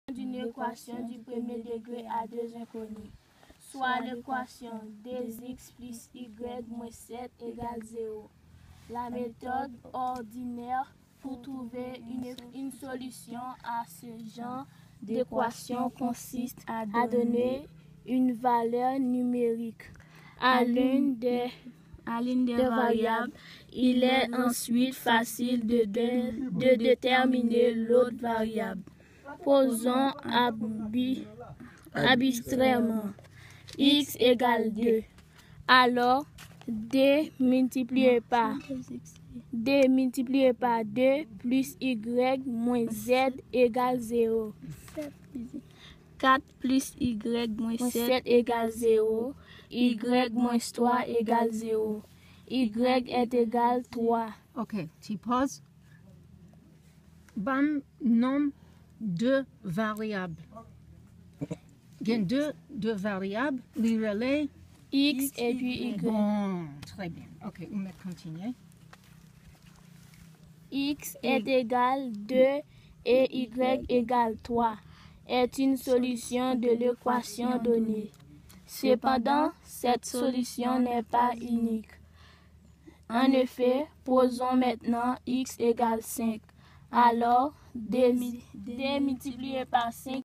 NOTHING WILL STOP US! Nou etidye Algebre
Two young neighbors join me in deciphering some algebra problems in preparation for 9em ane a lekol - No matter what, we will solve for Ix and Ygret and more!